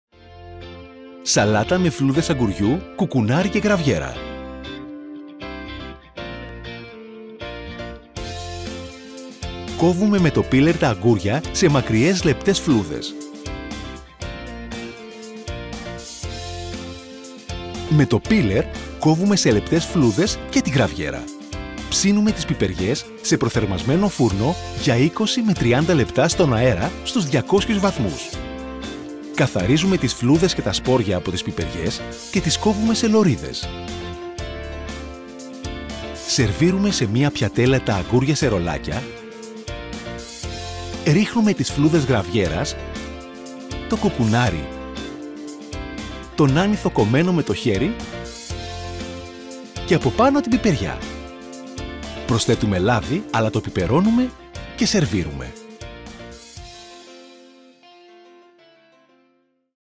Ensalada con virutas de pepino, piñones y queso Graviera (ES)
Salad with cucumber peels, pine nuts, and graviera cheese